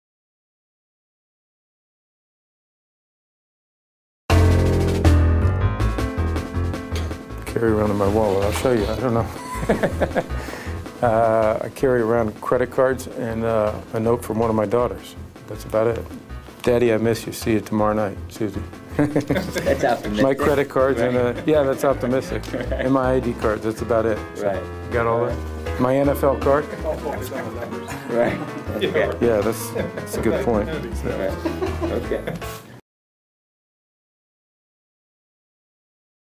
In Your Wallet: NFL Commish Roger Goodell
Ever wondered what's in a bigwig's wallet? NFL commissioner Roger Goodell was kind enough to give us a tour of his surprisingly scant billfold that was better measured by a message from his daughter. (Sept. 17)